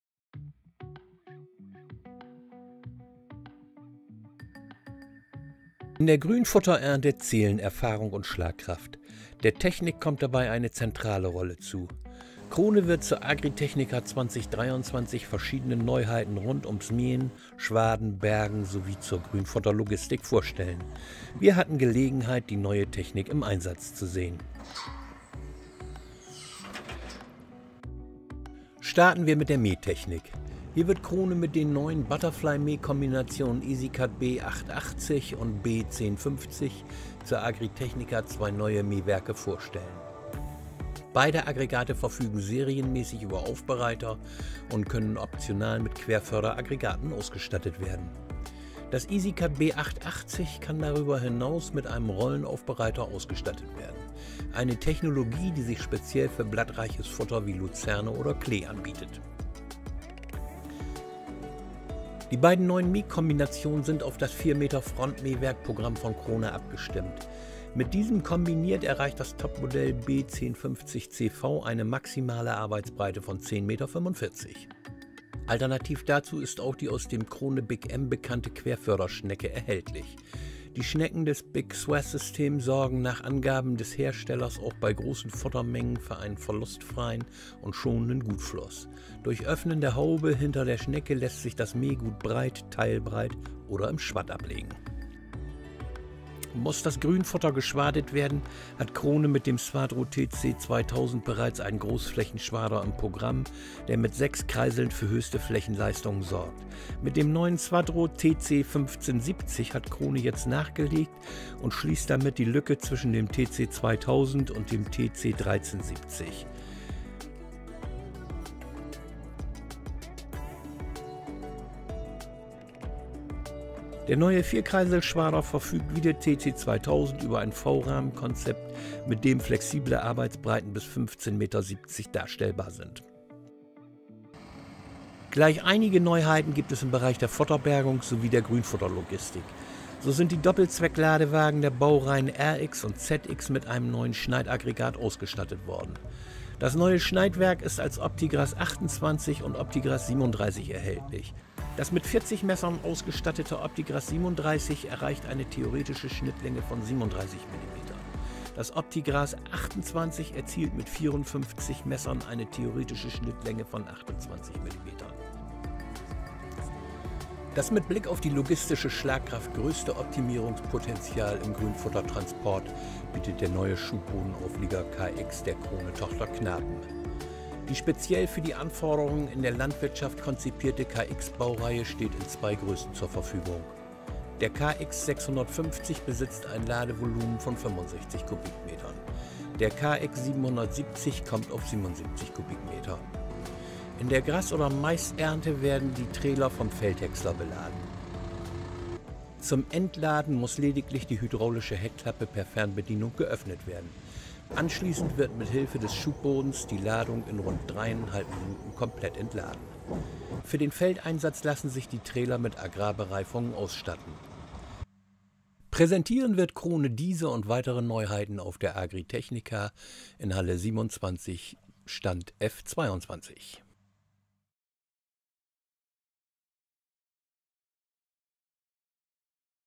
Krone Neuheiten 2024: Innovative Grünfuttererntetechnik
Neue Mähtechnik, ein weiterer Großflächenschader, überarbeitete Ladewagen oder ein spezielle auf die Bedürfnisse der Agrarbranche konzipierter Schubbodenauflieger: Krone wird auf der Agritechnica 2023 zahlreiche Neuheiten für die Grünfutterernte präsentieren. Einige davon konnte wir bereits im Praxiseinsatz sehen.

Bild Schnitt und Ton: Stephan Keppler
Redaktion LOHNUNTERNEHMEN

Beckmann Verlag GmbH und Co. KG
Rudolf-Petzold-Ring 9
31275 Lehrte